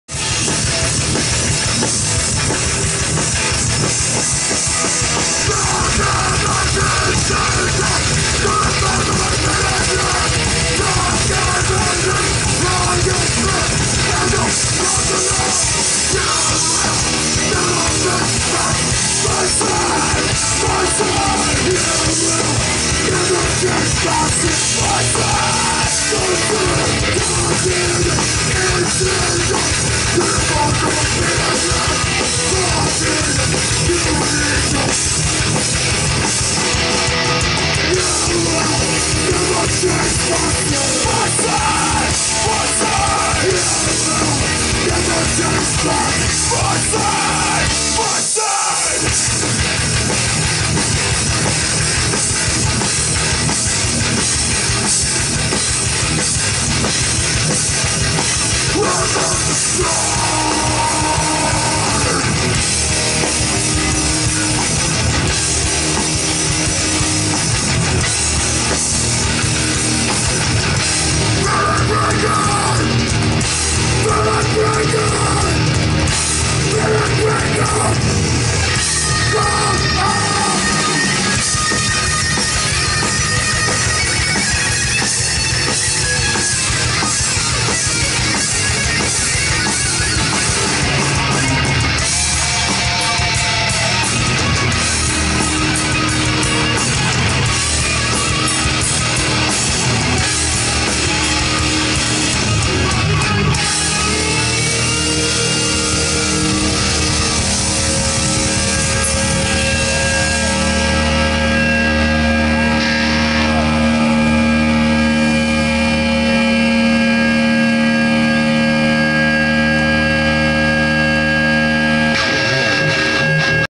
Integrity (Amnesia Rockfest 2016)
This is Integrity playing at the Amnesia Rockfest in Montebello, Quebec, Canada on June 24, 2016.